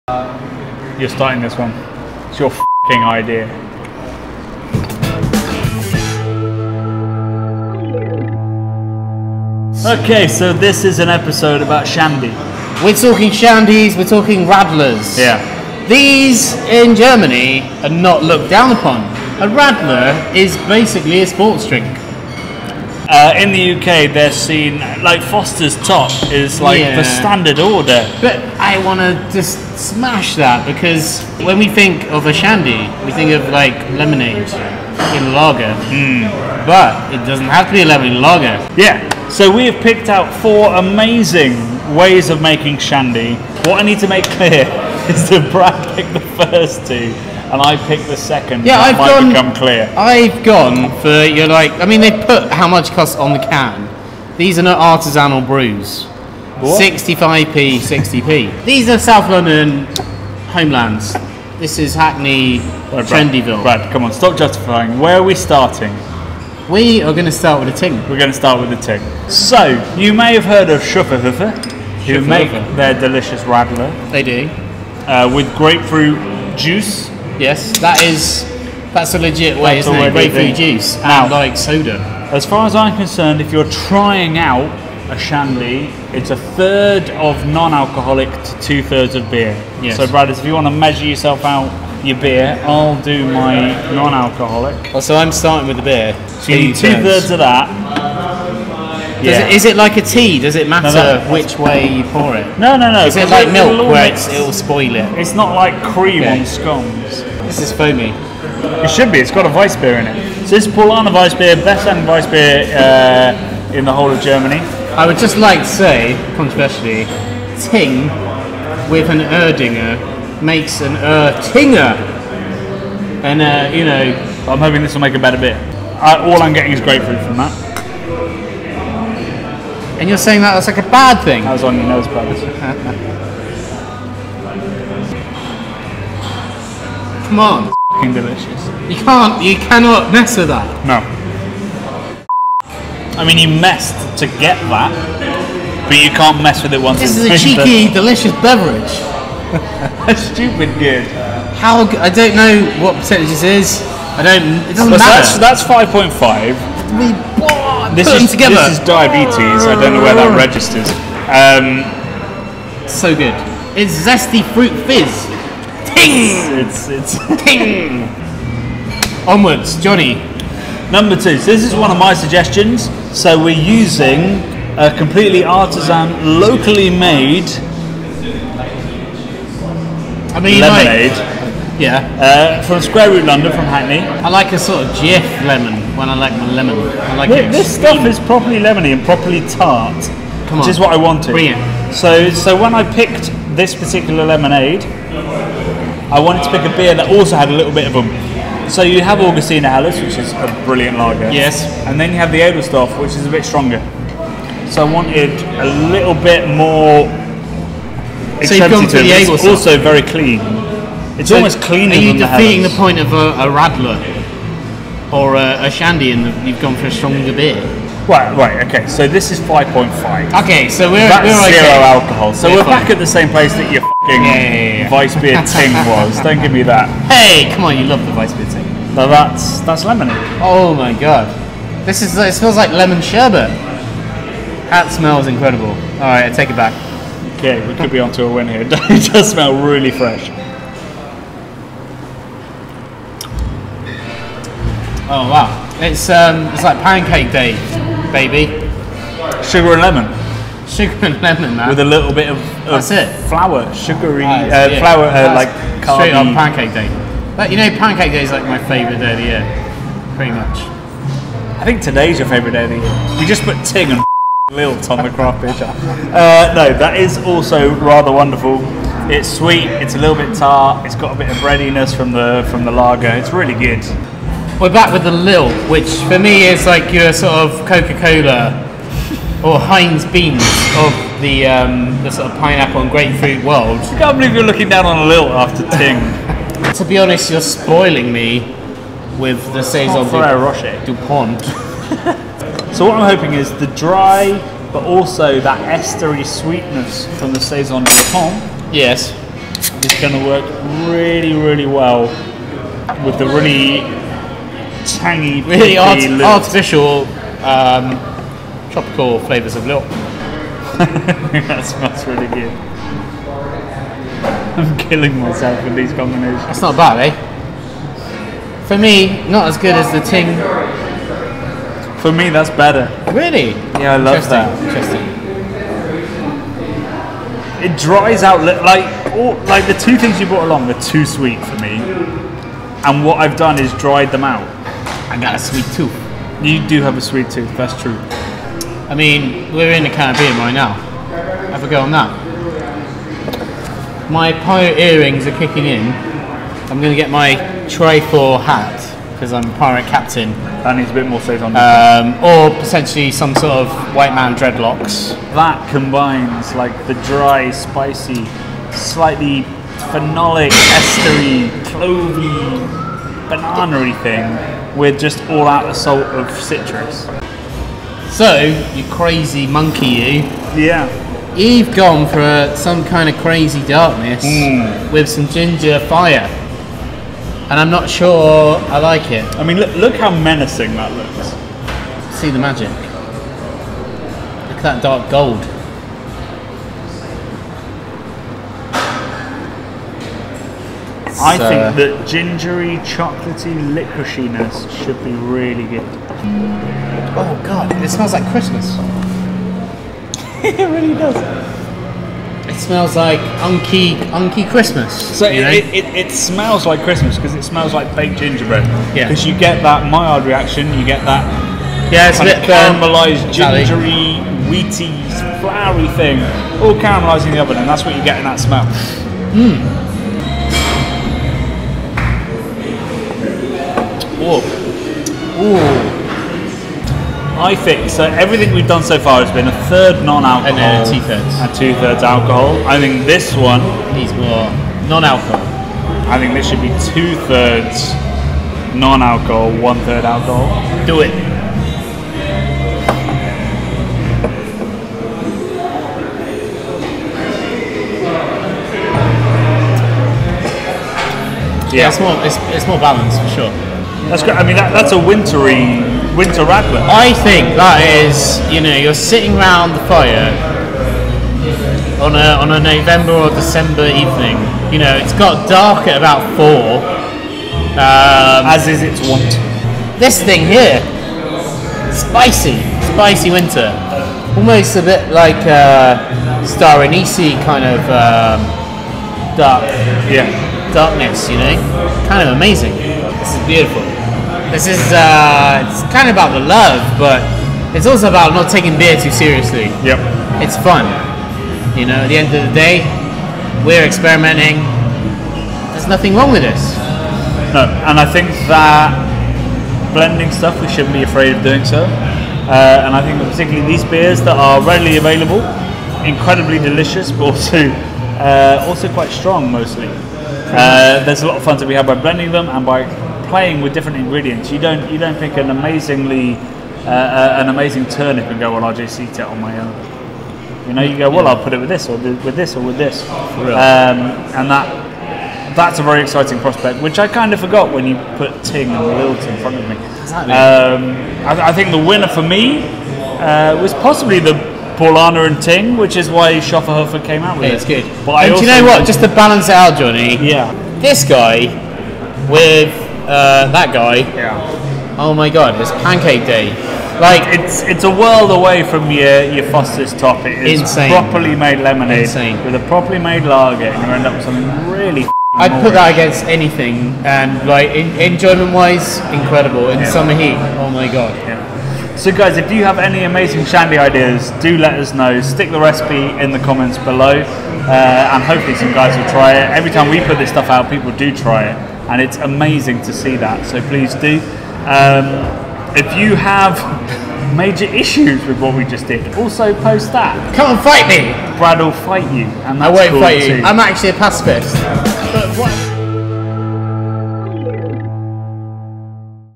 FOUR Amazing Craft Shandies (summer sacrilege or saviour?) | The Craft Beer Channel
I think the weather got to us. The UK is going through an unprecedented heatwave and Jonny and Brad weren't built for the heat. Luckily though, they have a sweet way to stay hydrated...while also committing crimes against craft beer – artisan shandy.

Seriously though, some shandies are awesome and we have come up with four of the best. Sorry.

Buy our new book, the London Craft Beer Guide, today:

Remember to drink responsibly(ish) and not be that guy...